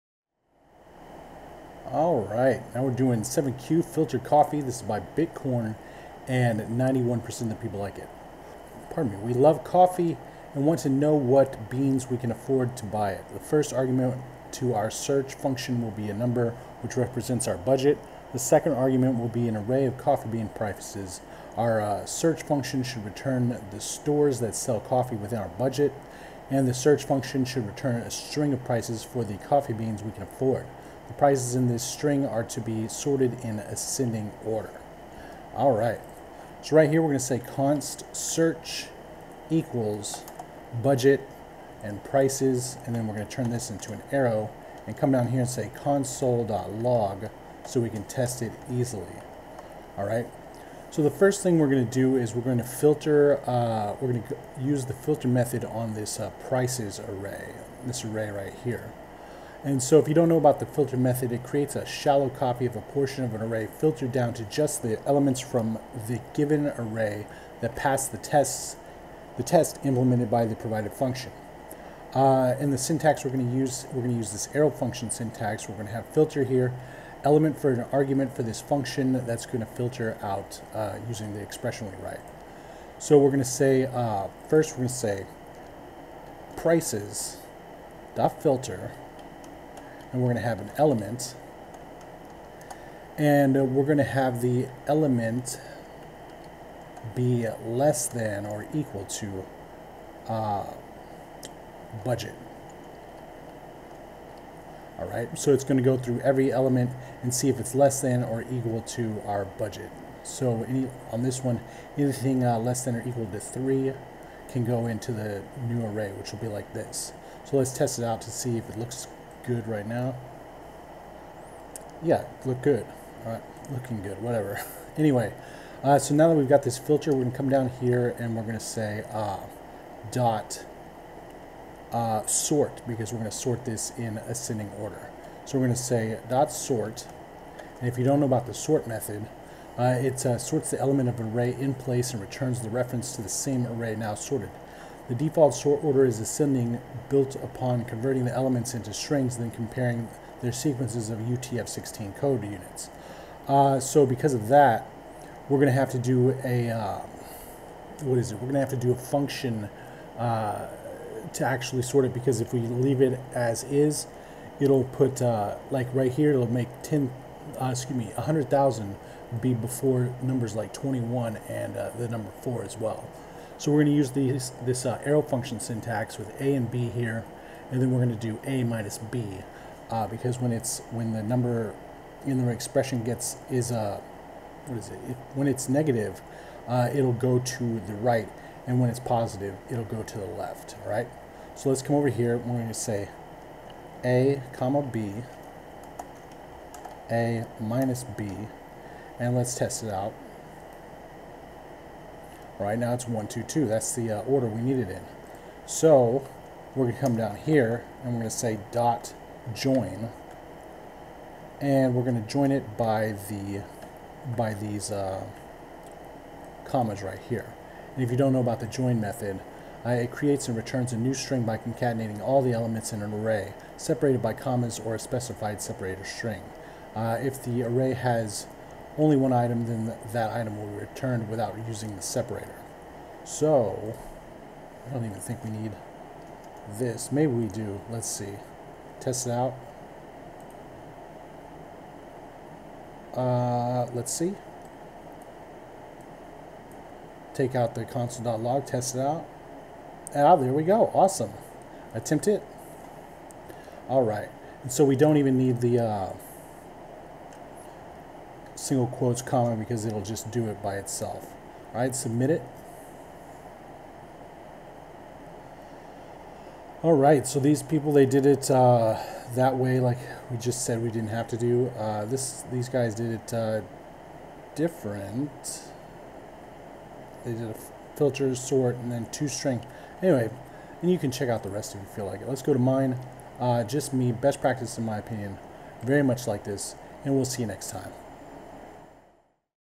Codewars 7 kyu Filter Coffee Javascript
this one uses the filter(), sort(), and join() methods